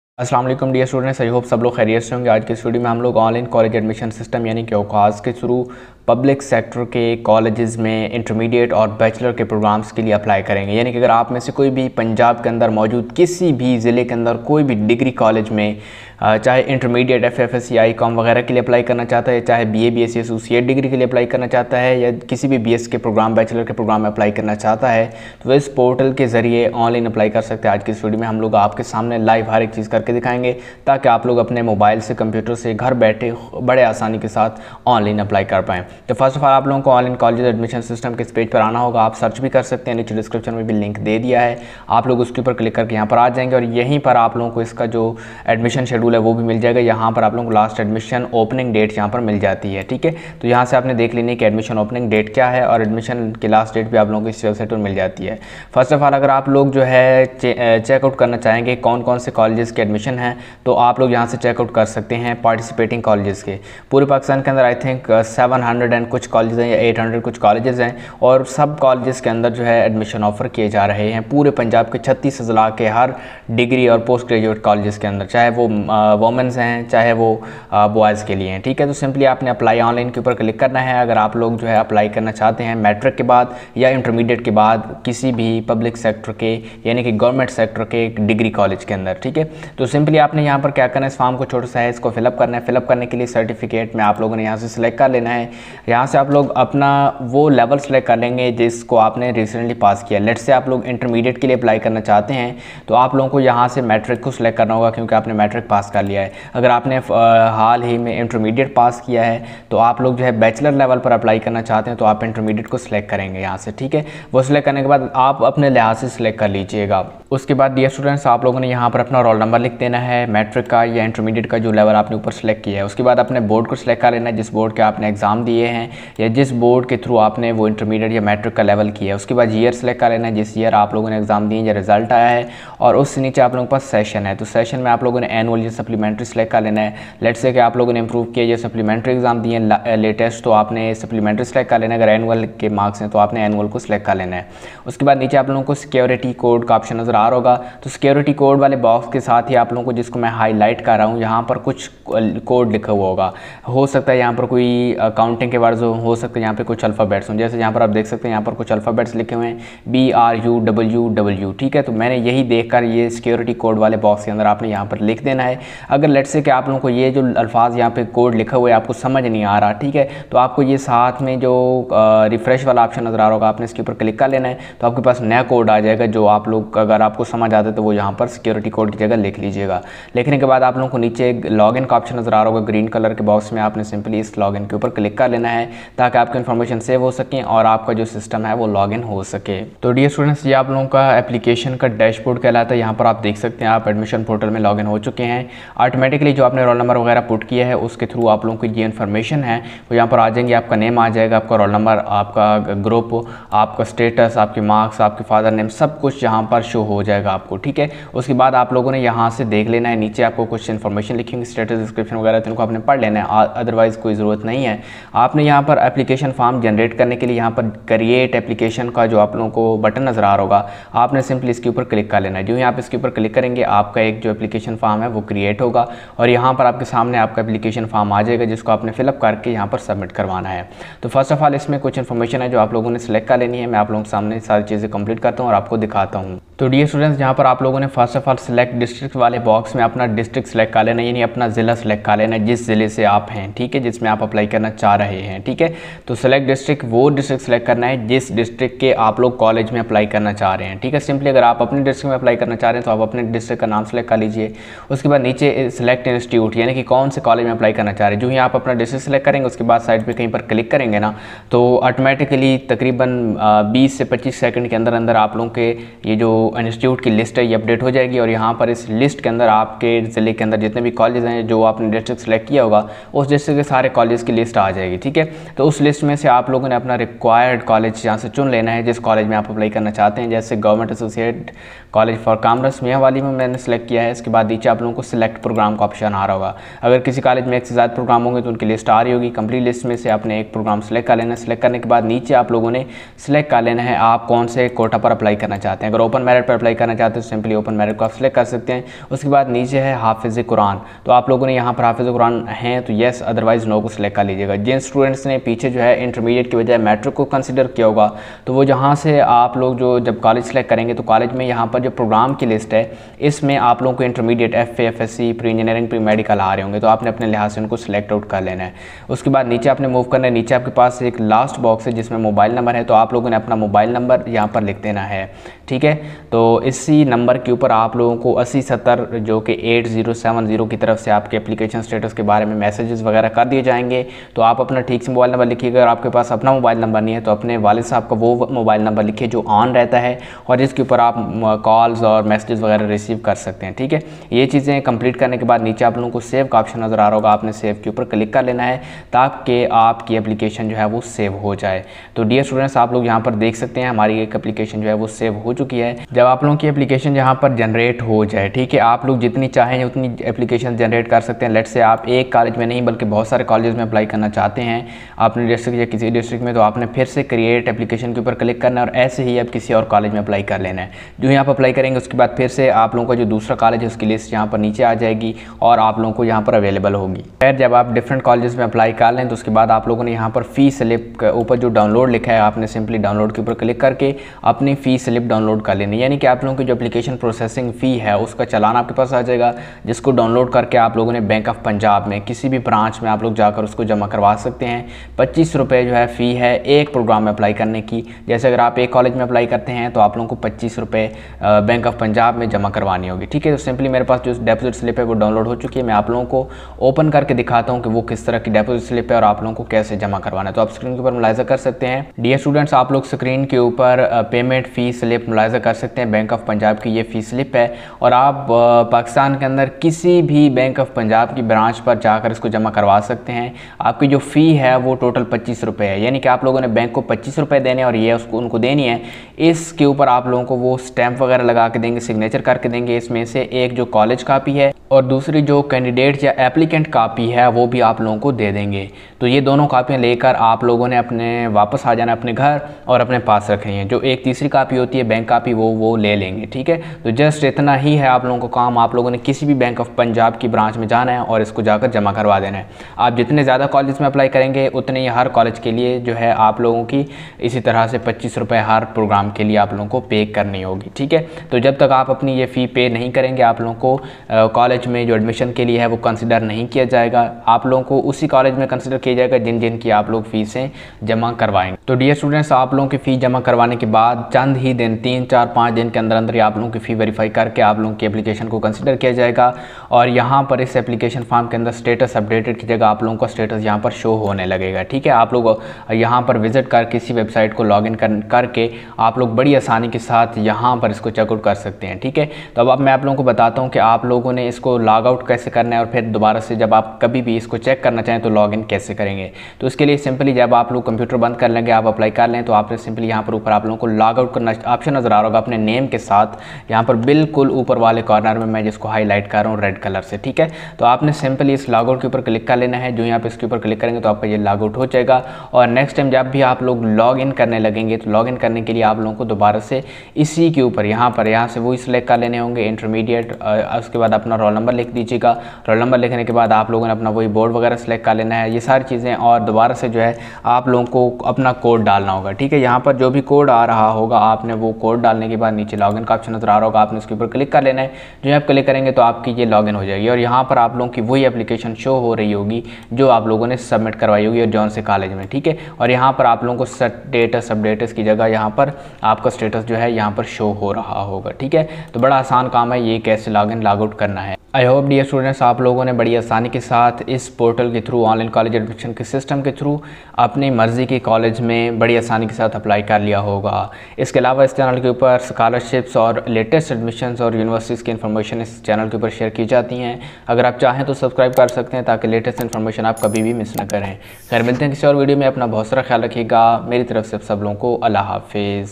How to apply for admission in Government Degree Colleges in Intermediate (11th) & Bachelor's (ADP)
How to apply for Admission in Govt Degree & Postgraduate (pg) Colleges by Online College Admission System (OCAS). In this video we have shown how to apply in Government Degree Colleges for admission in Intermediate (FA/FSc/ICS/ICom) after matric & Bachelor or Associate Degree (ADP/ADS/ADC/ADCS/ADA/BA/BSc) after Intermediate. All Punjab Government Degree Colleges online admission form filling procedure is given here step by step.

Important Links

How to Apply in Punjab University Online
** Latest Video is under process (Coming Soon)